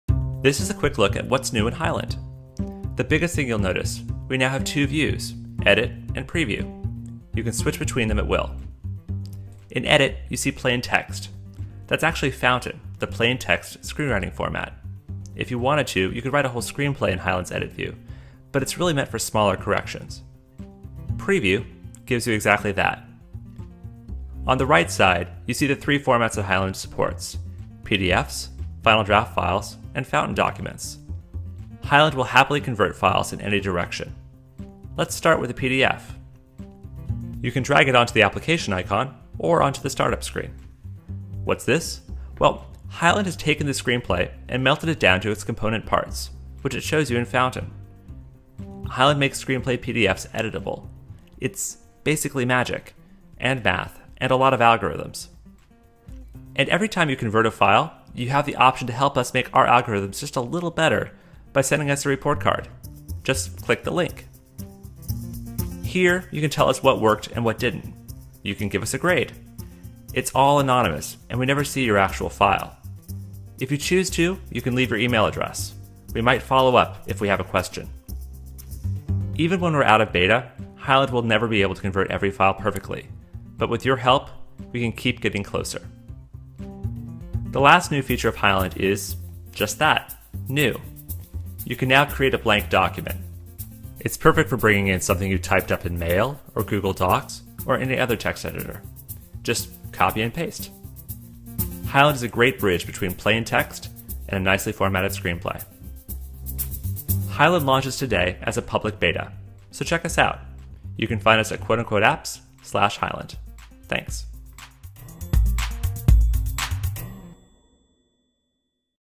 Highland Beta Screencast
Highland is new utility for Macintosh that makes it easy to convert screenplay files between the three main formats: PDF, .fdx and Fountain.